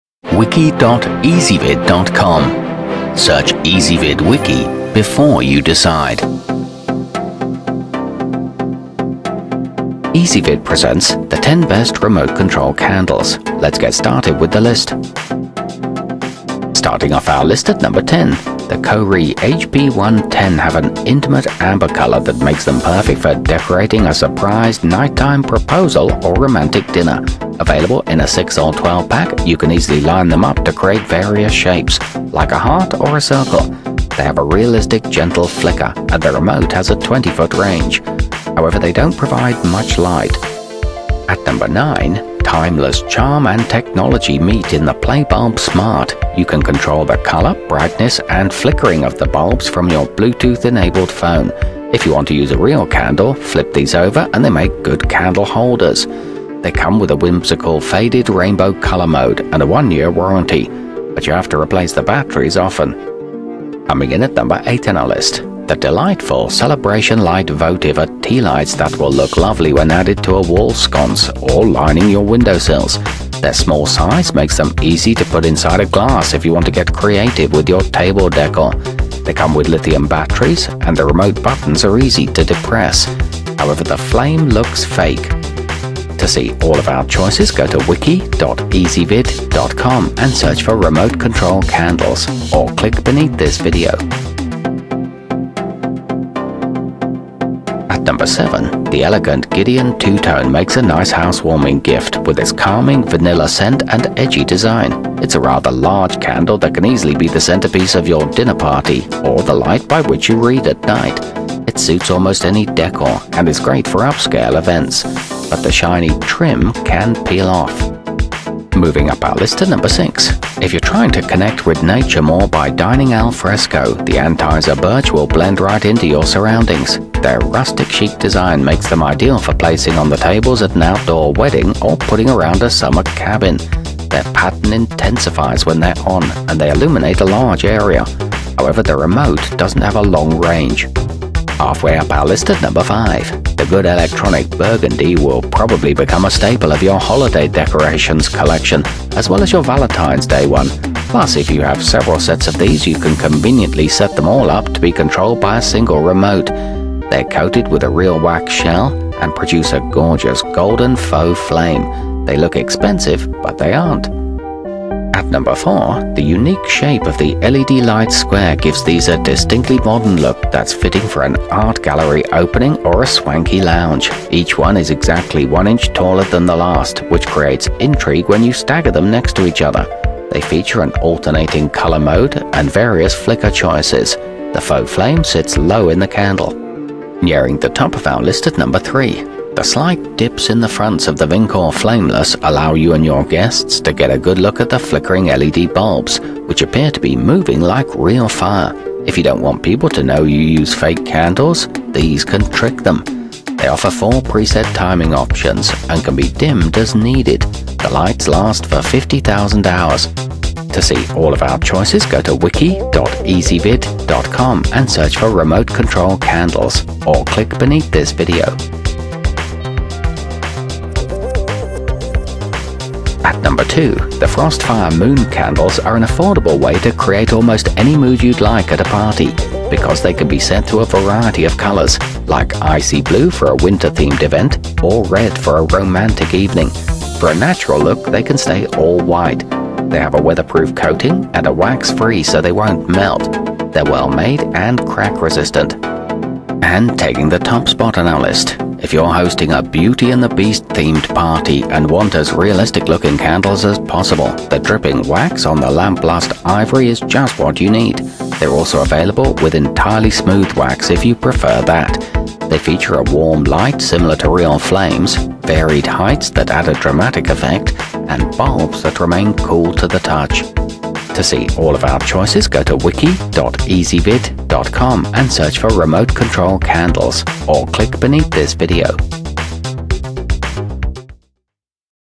10 Best Remote Control Candles 2017
Please Note: Our choices for this wiki may have changed since we published this review video. Our most recent set of reviews in this category, including our selection for the year's best remote control candle, is exclusively available on Ezvid Wiki.

Remote control candles included in this wiki include the lamplust ivory, celebrationlight votive, vinkor flameless, antizer birch, kohree hp110, playbulb smart, led lytes square, frostfire mooncandles, gideon two-tone, and good electronic burgundy.